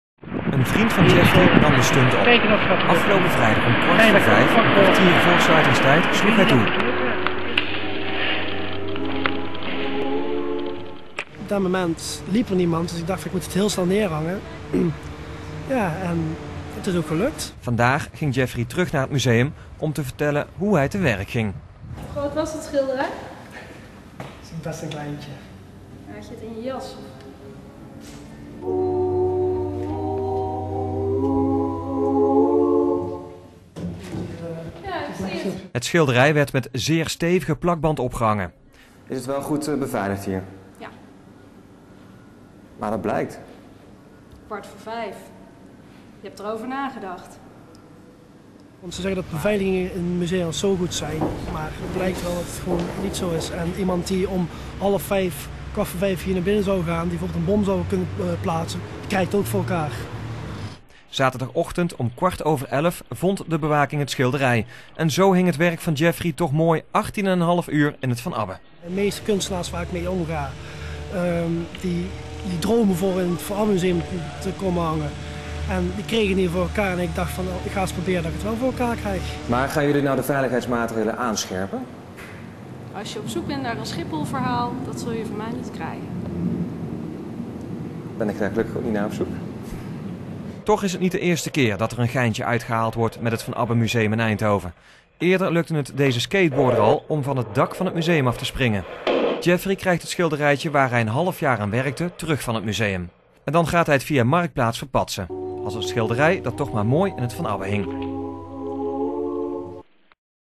Man hangt eigen schilderij op in Van Abbemuseum
Een Eindhovense man heeft vrijdag 15 februari zijn eigen schilderij in het van Abbemuseum gehangen. Het werk werd pas een dag later ontdekt door de bewaking en weggehaald.